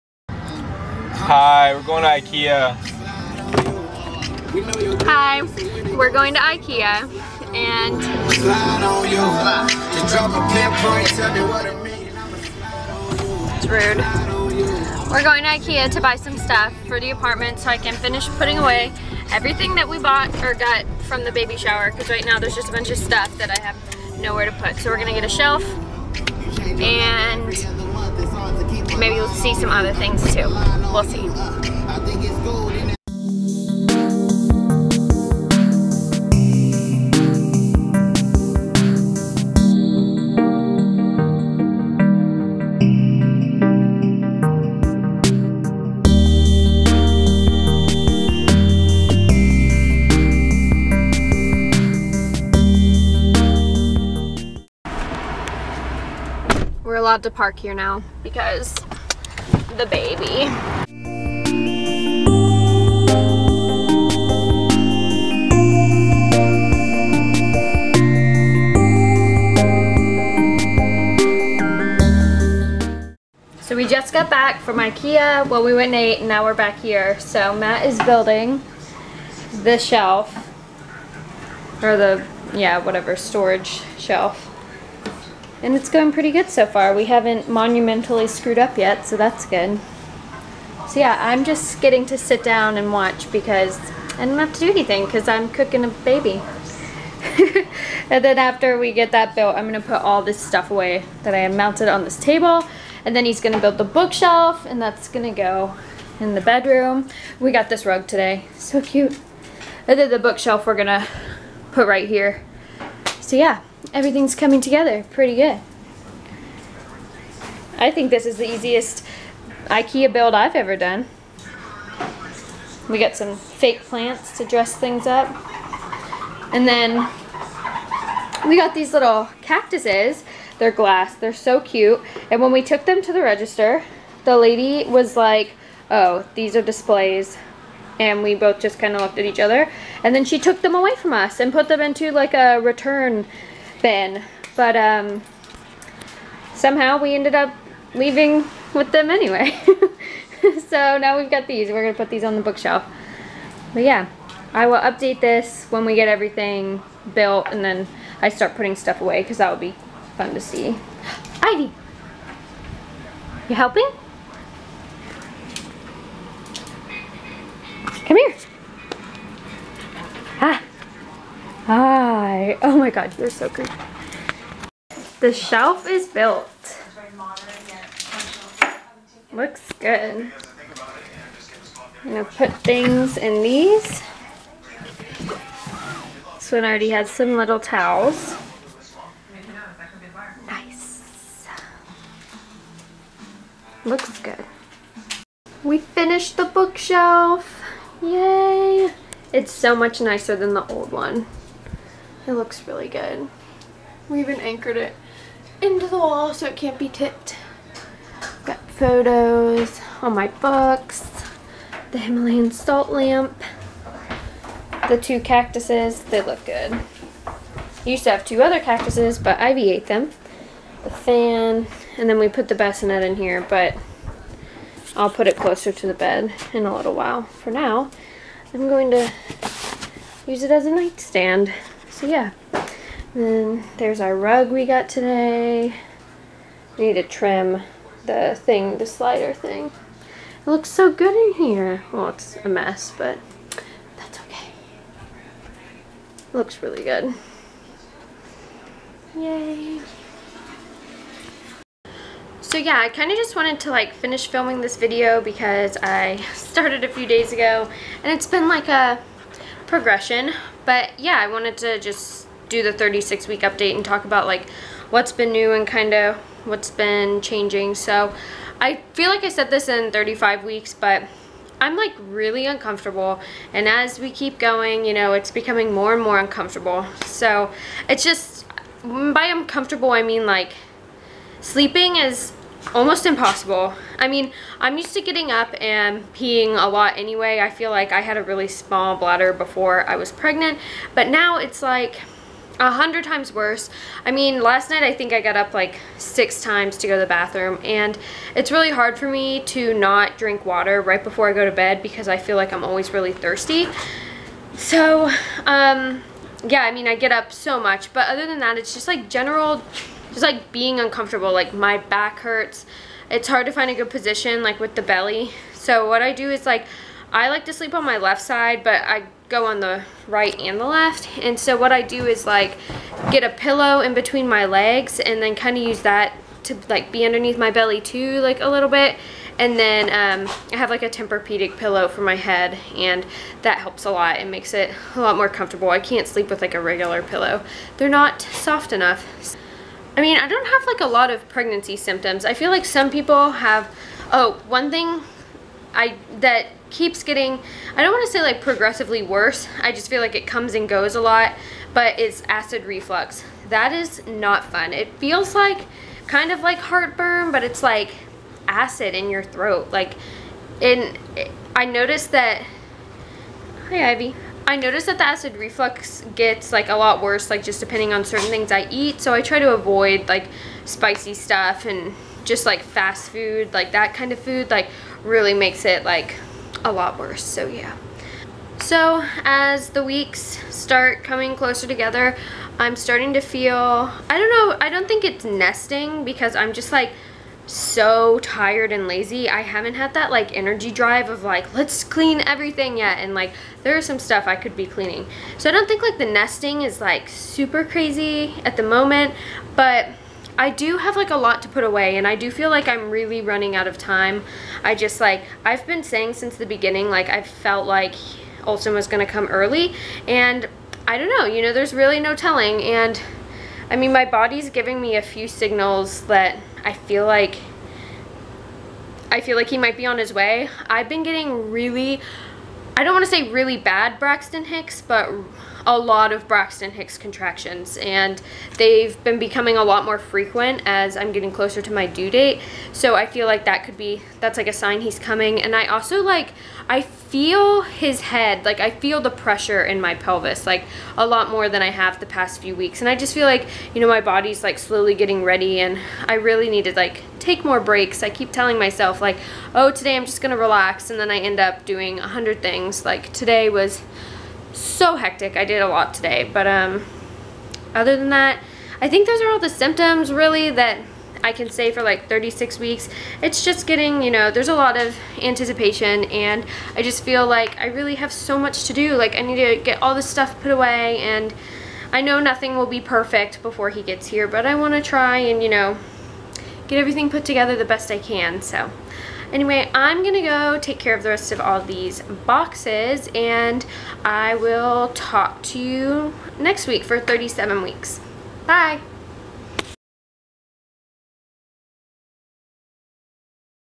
36 Week Pregnancy Update! Ikea Shopping For Baby!
This week we went out and got some more stuff to put our apartment together. 36 weeks really started to weigh on me, and sleeping through the night is now a thing of the past. I can't wait to meet this sweet boy and finally see the face I have been picturing for so many months.